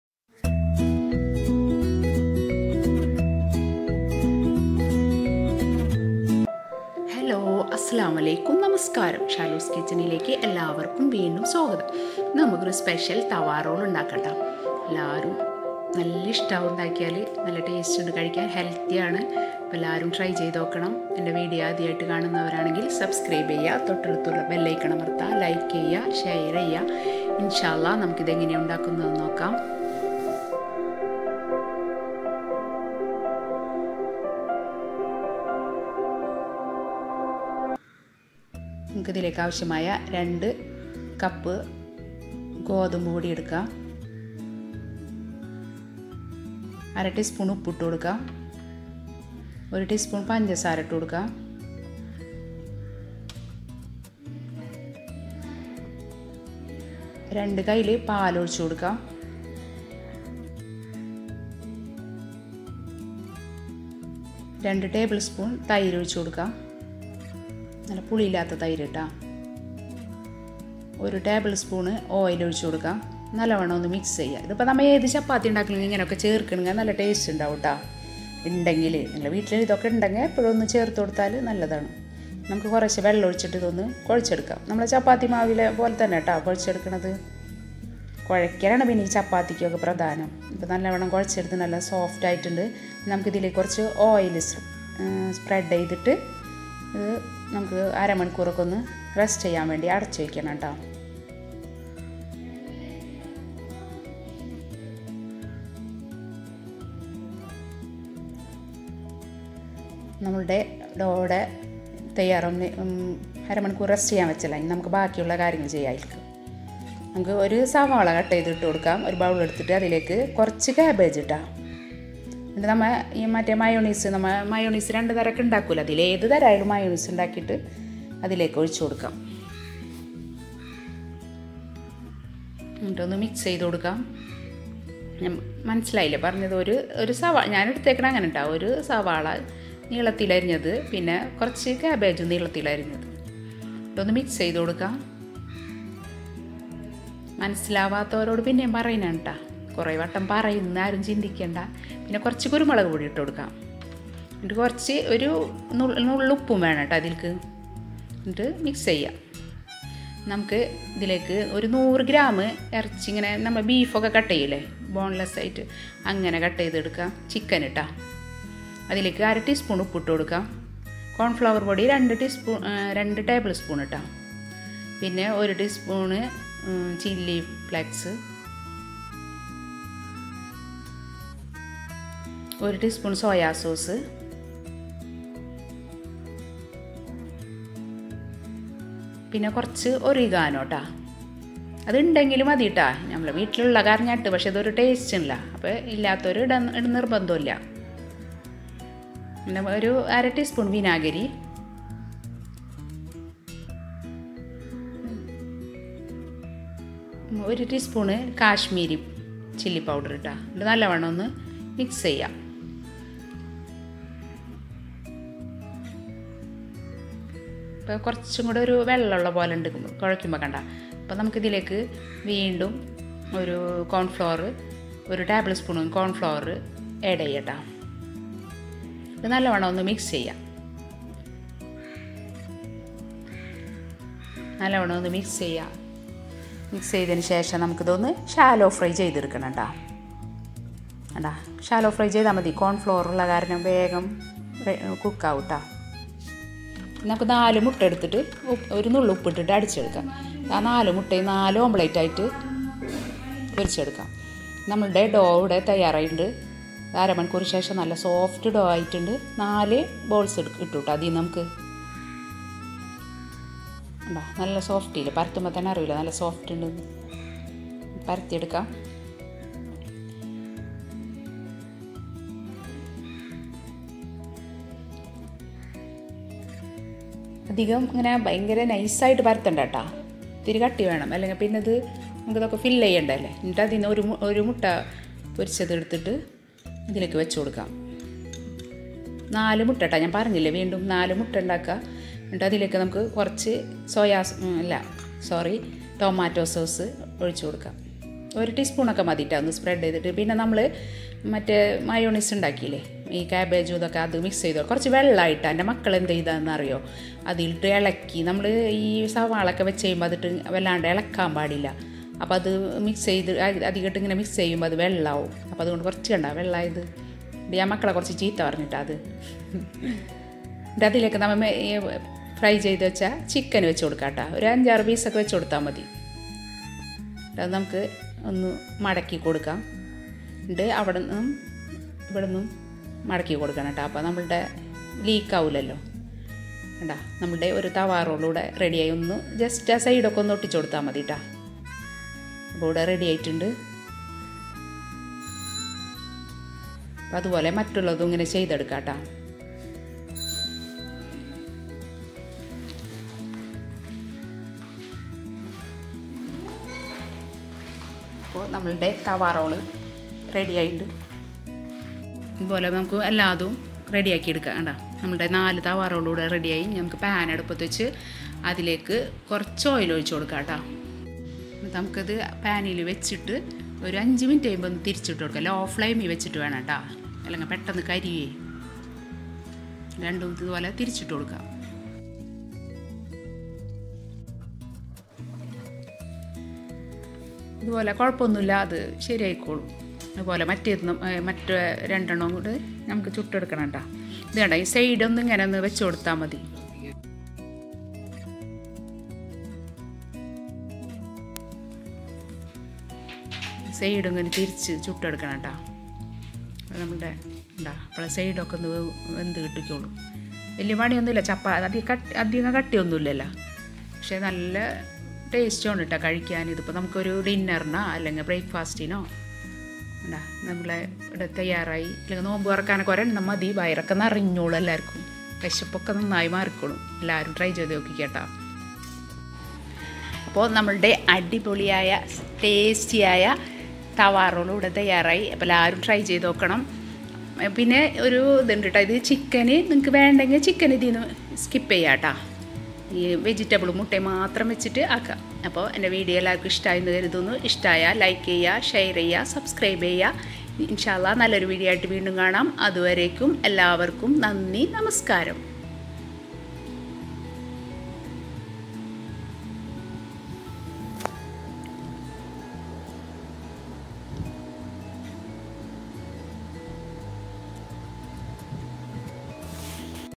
ഇതൊരെണ്ണം മതി || TAWA ROLL || വയറും നിറയും മനസ്സും നിറയും 🤤🤤 (shalus tasty kitchen )r. No=23
THAWA ROLL

INGREDIENTS:

wheat : 2 cup
curd : 2 tablespoon
milk : 2 tablespoon
oil : 1 tablespoon
sugar : 1 teaspoon
salt : 1/2 teaspoon
chicken : 100g
chilli flakes : 1 teaspoon
onion : 1 piece
cabbage : 50g
oregano : 1 teaspoon
soya sauce : 1 teaspoon
vinegar : 1/2 teaspoon
Kashmiri chilli : 1/2 teaspoon
salt : 1/2 teaspoon
corn flour : 3 tablespoon
egg : 4
mayonnaise